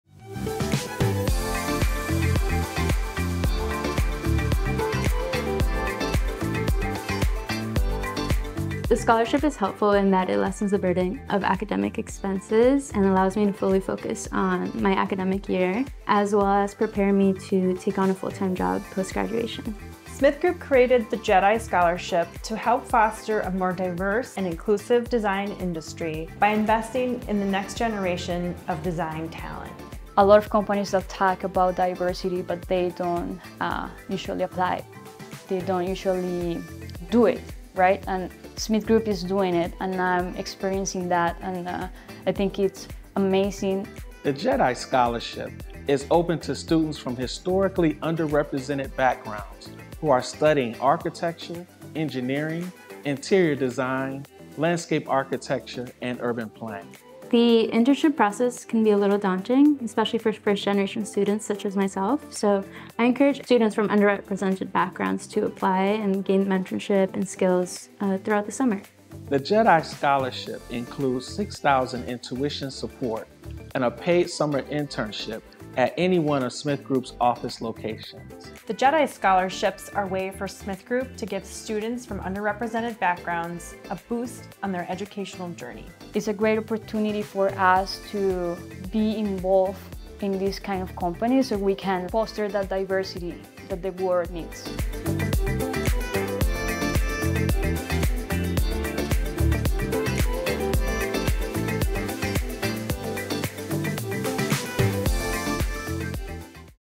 CLOSED | Applications for 2024 Justice Equity Diversity and Inclusion Scholarship
Our 2024 Justice, Equity, Diversity and Inclusion scholarship application is now closed. We look forward to announcing our selected scholarship recipients in the spring.

Scholarship applications are opened annually in the fall for students from historically underrepresented backgrounds studying architecture, engineering, interior design, landscape architecture or urban planning.